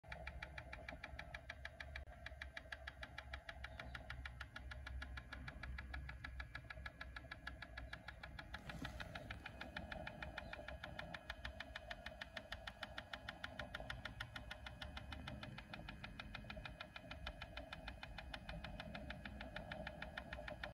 Simpson Desert Sunset Themed TroyCraft Custom Shaving Brush
I was asked to make a shaving brush in the theme of a sunset in the Simpson Desert. Shown here with a high quality 26mm 2 band badger bristle knot.